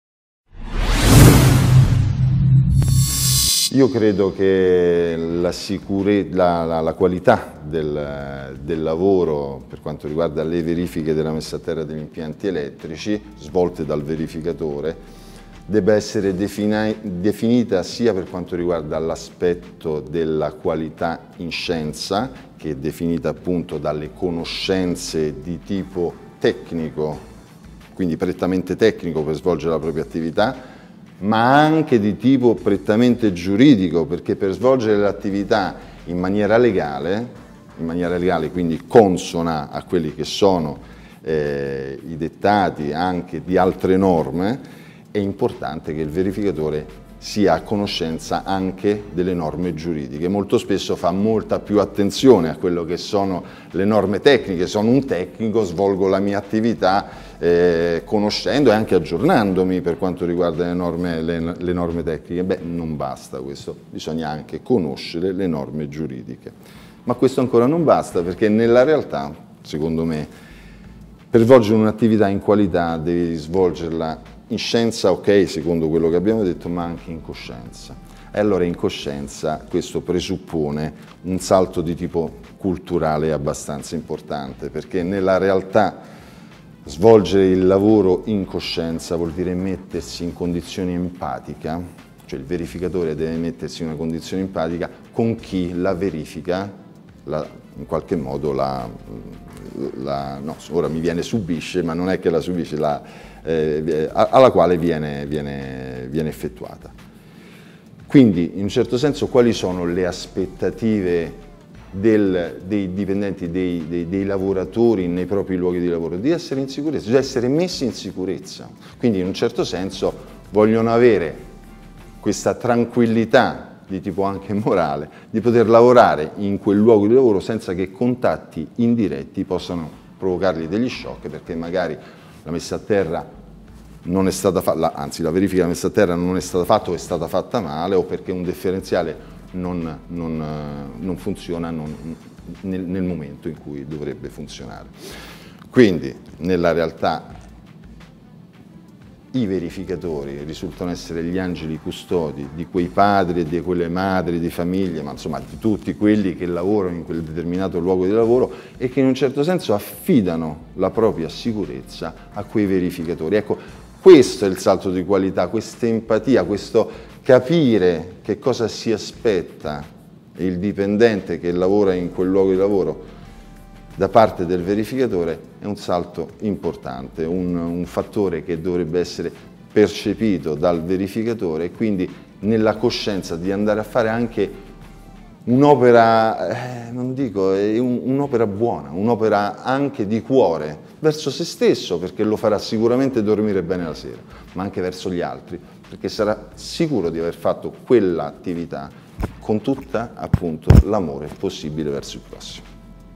Intervista a Riccardo Chiesi. Forum Sicurezza 2015
L'intervista a Riccardo Chiesi, Funzionario del Ministero dello Sviluppo Economico intervenuto in occasione del convegno inaugurale del 28 aprile 2015.
Intervista di Raffaella Bucci.